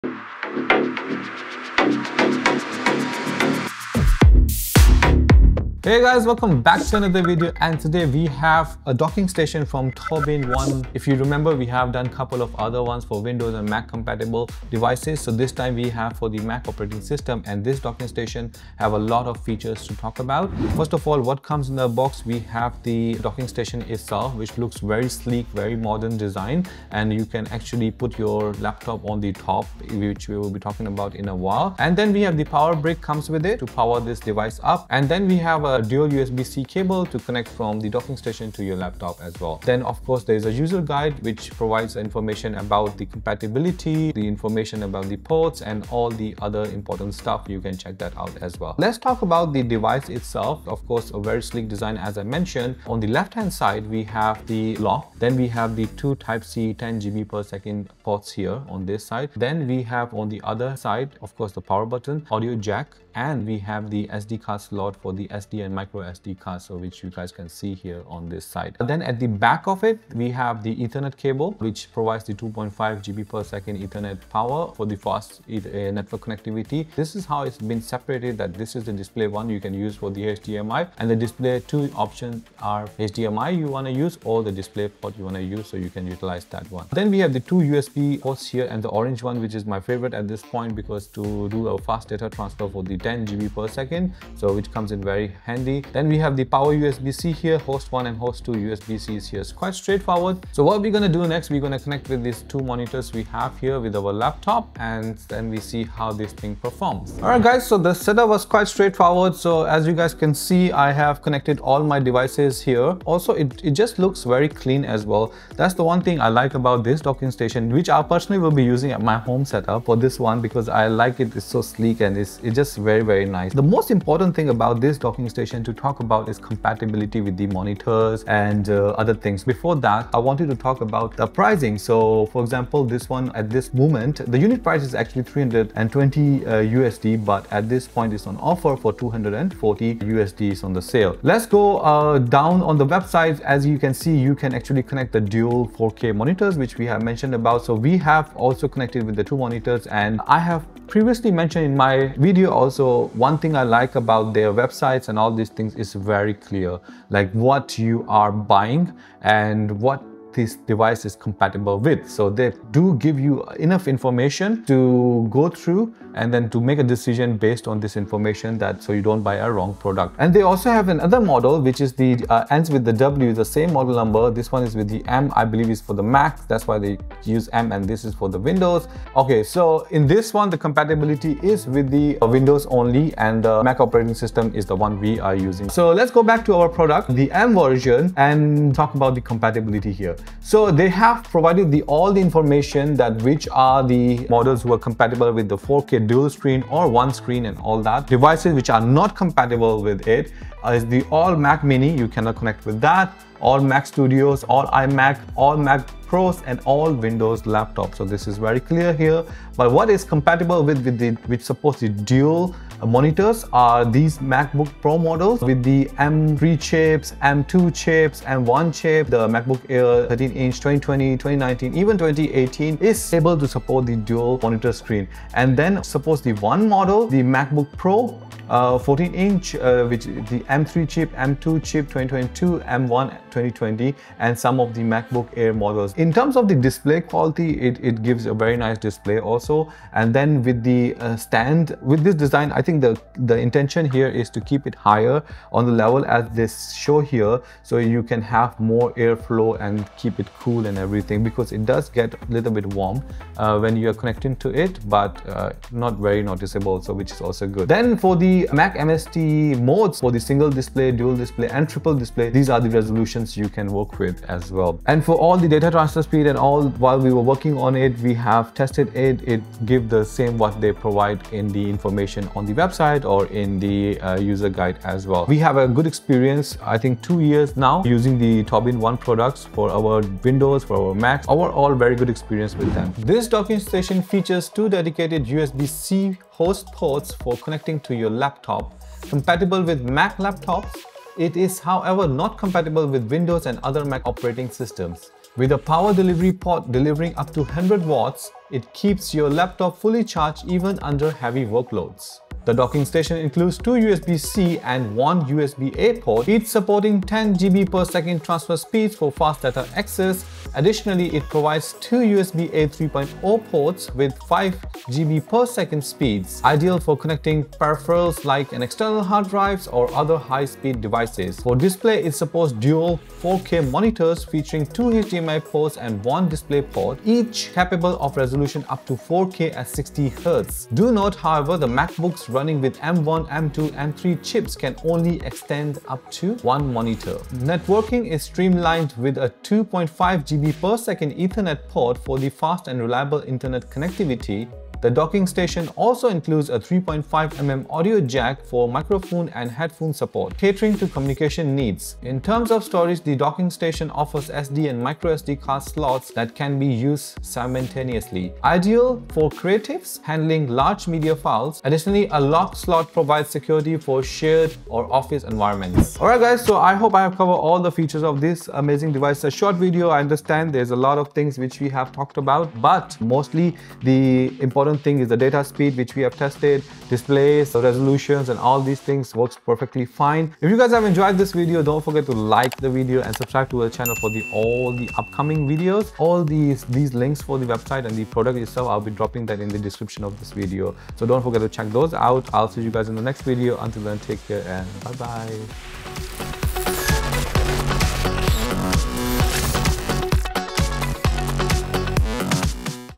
TobenONE UDS041M: The All-in-One Dock for Multitasking
In this video, we review the TobenONE UDS041M USB-C Docking Station—a versatile hub designed to expand your Macbooks connectivity. With dual 4K display support, 100W power delivery, and high-speed USB ports, it’s perfect for professionals and creatives seeking seamless performance.

Key Features:
Dual 4K Display Support: HDMI and DisplayPort up to 4K at 60Hz
High-Speed Ports: USB-C and USB-A ports with 10Gbps transfer speeds
Reliable Ethernet: 2.5Gbps Ethernet port for stable connectivity
Power Delivery: 100W USB-C charging capability

0:00 - Intro
0:21- What’s In The Box?
0:50 - Device Overview
1:47 - Putting it to the test & Product Information
4:43 - Specs & Features
6:06 - Outro